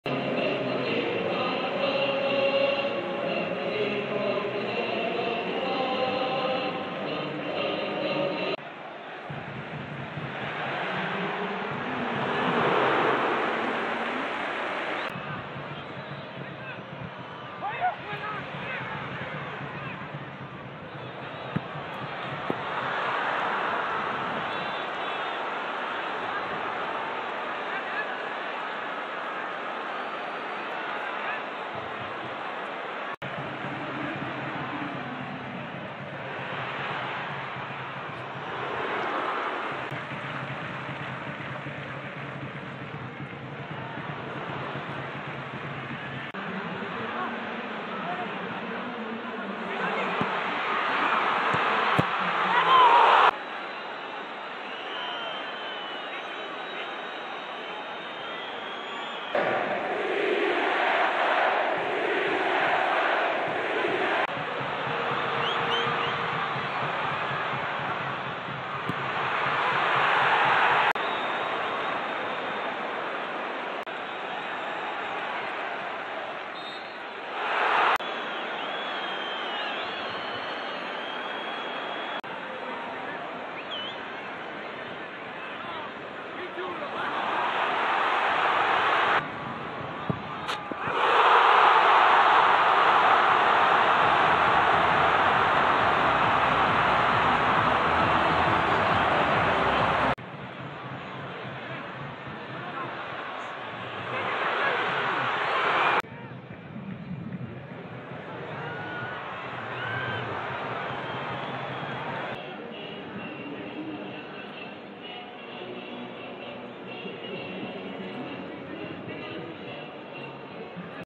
Resumen de UD Las Palmas vs Real Sporting (1-1)
Empate a uno entre UD Las Palmas y Real Sporting con los goles de Queipo y de Jonathan Viera desde el punto de penalti LasPalmasRealSporting J33 LaLiga SmartBank 2022/2023